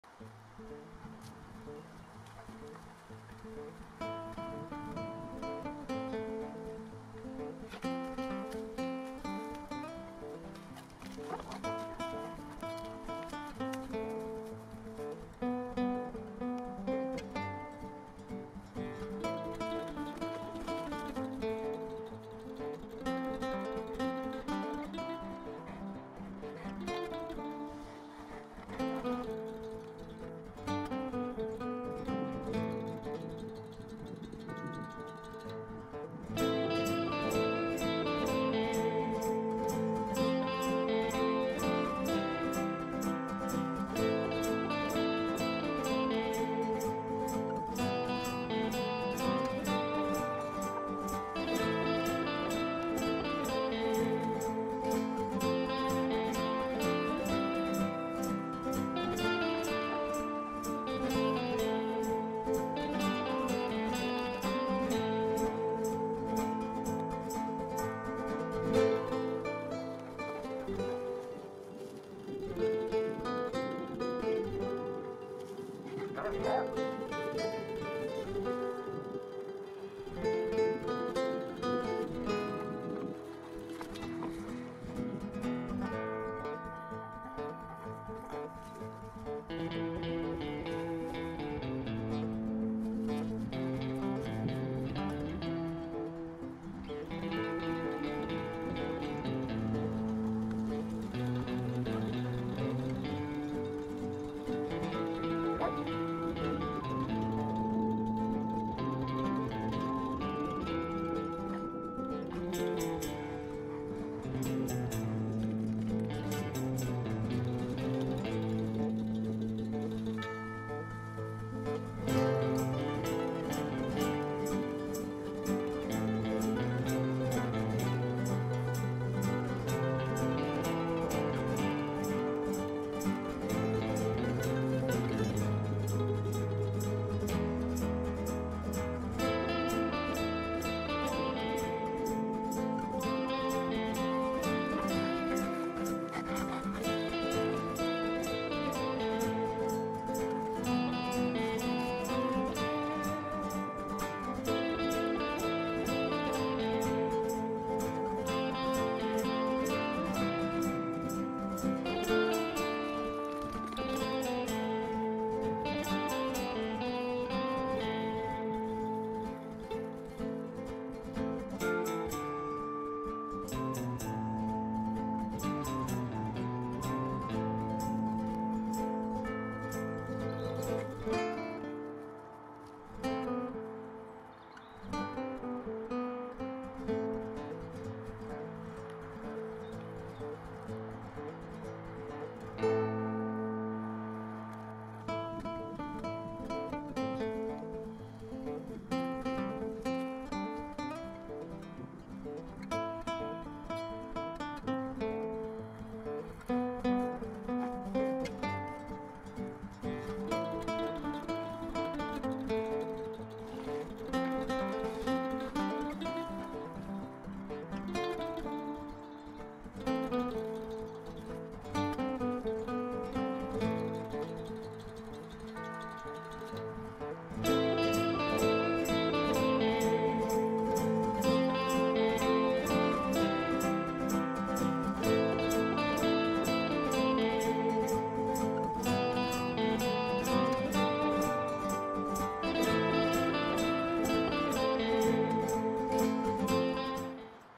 My Boerboel tries to knock me over
Boerboel Yzer is fooling around and having fun.